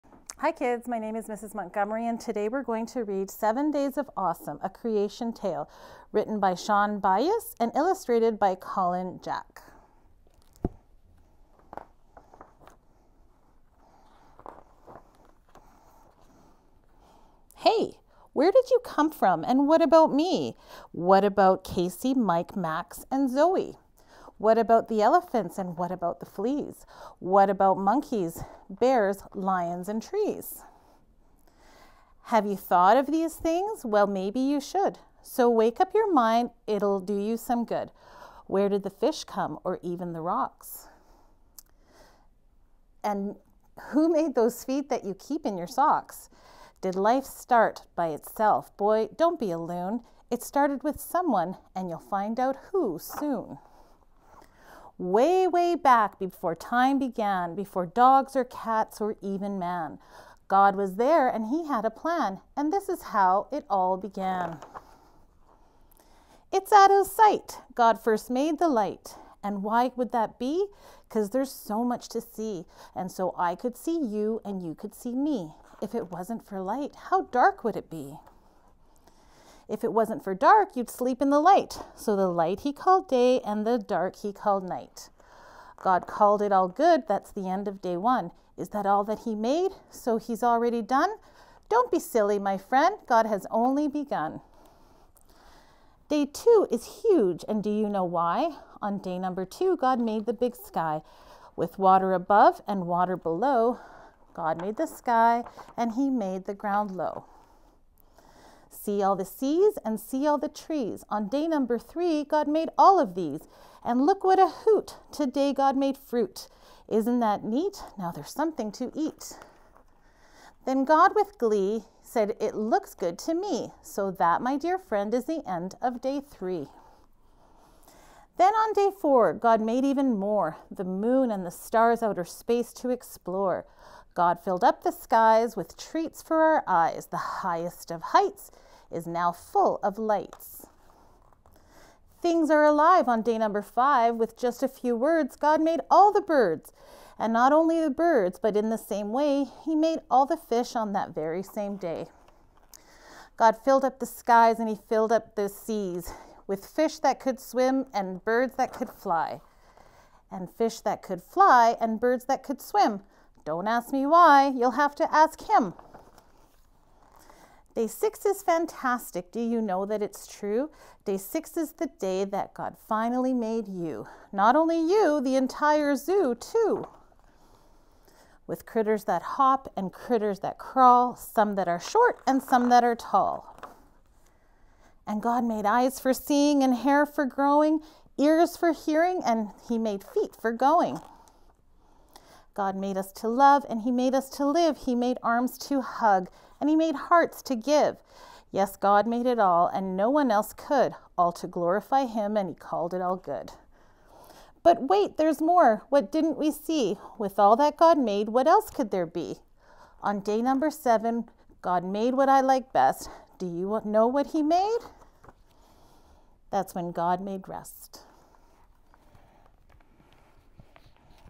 Story Time with Mrs. Montgomery
This week, Mrs. Montgomery is reading "7 Days of Awesome: A Creation Tale" written by Shaun Byous and illustrated by Colin Jack.